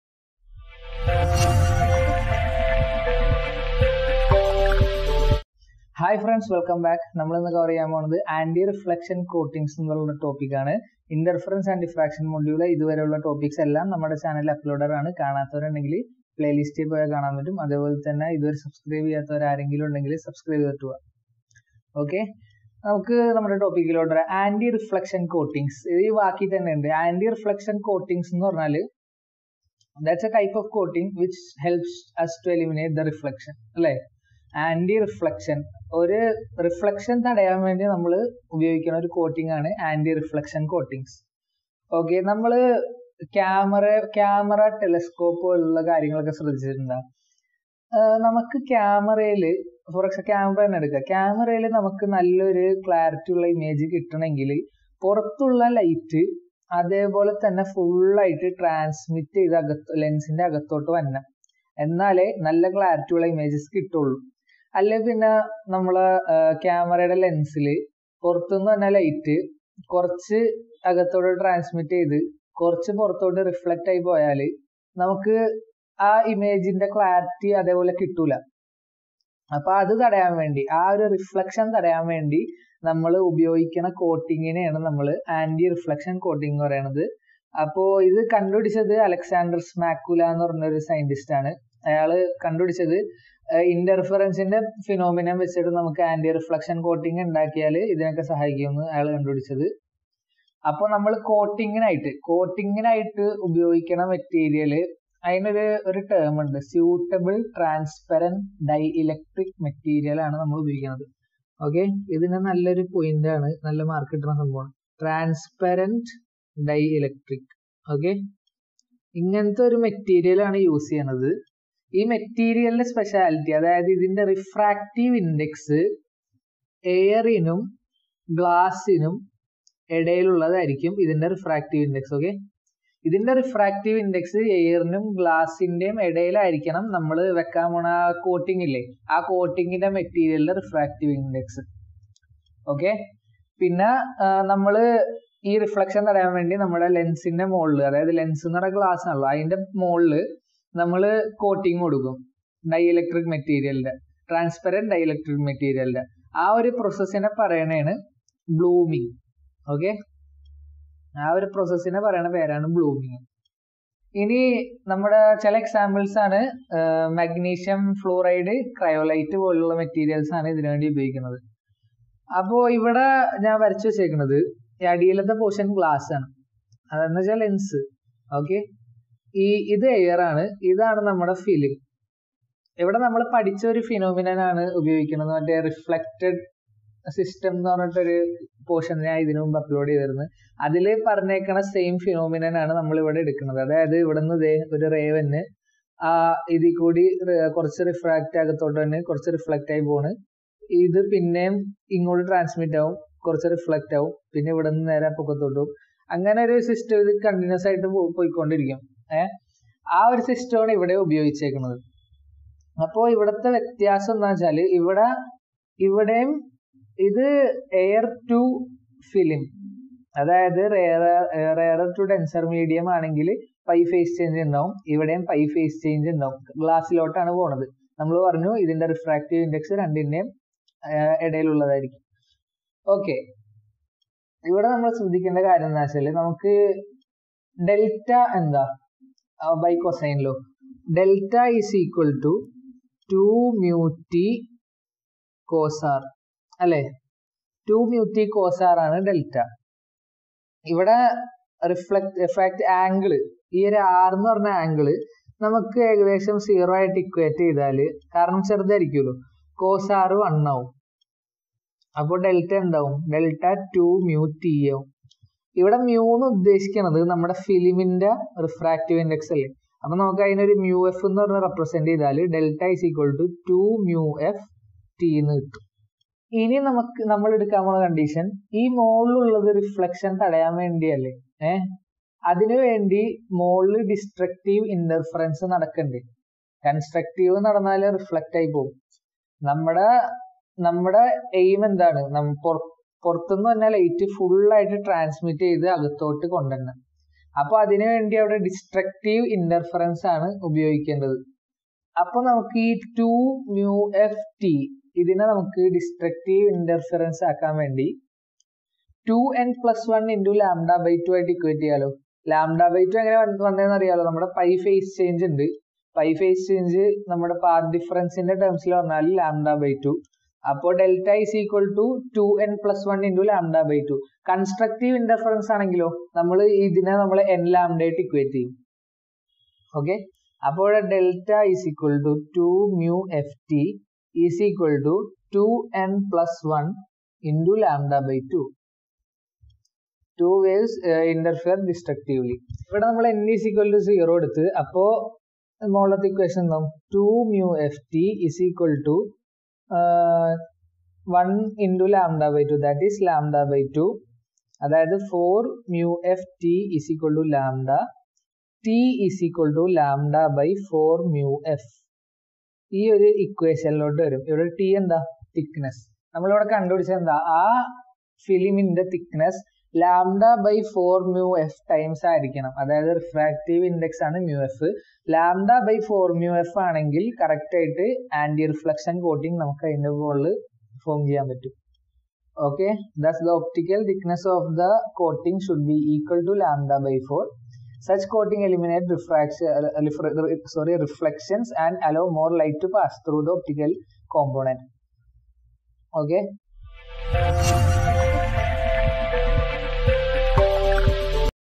Antireflection Coating | Interference and Diffraction | Engineering Physics | Module 2 |Target KTU
In this video you are about to learn the antireflection coating seen in cameras and other optical instruments which resist reflection and allows transmission of light.